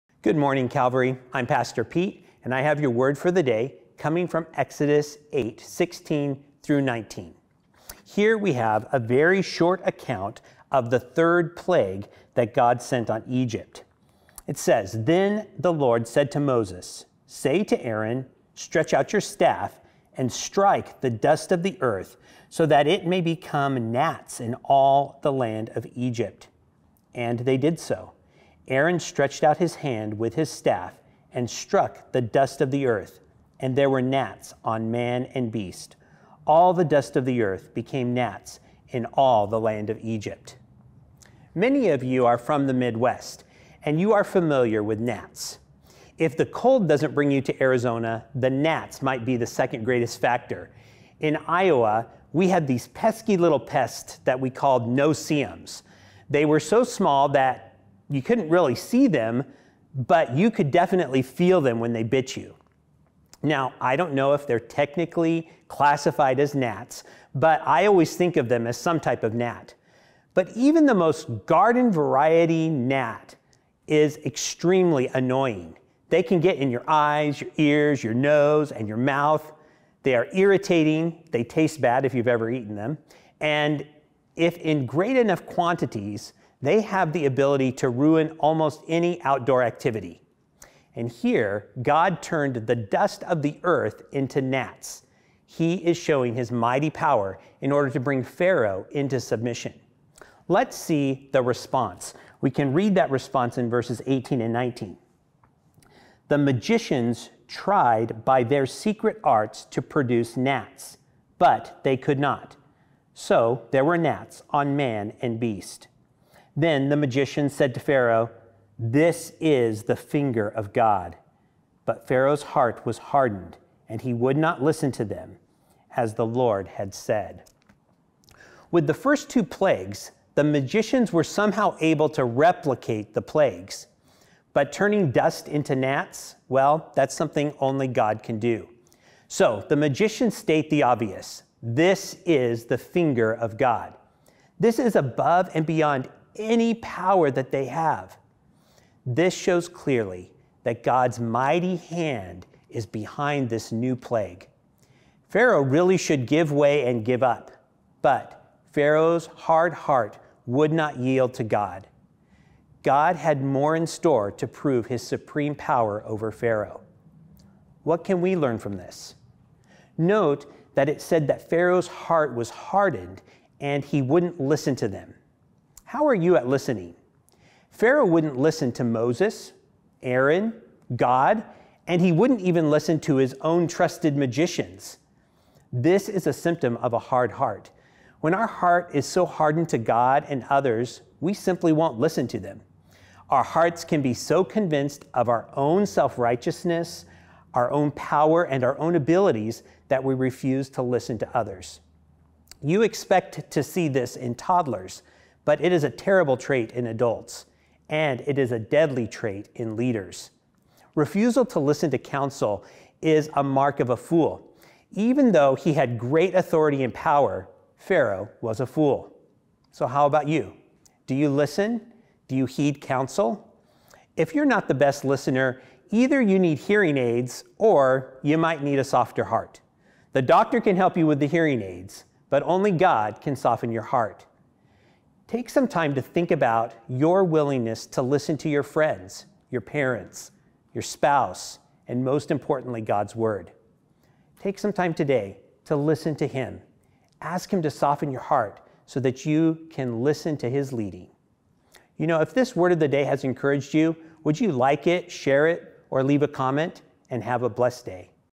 Your Word for the Day - Episode 496
Today Pastor Pete describes that even the awful plague of gnats wasn’t enough to soften Pharaoh's heart.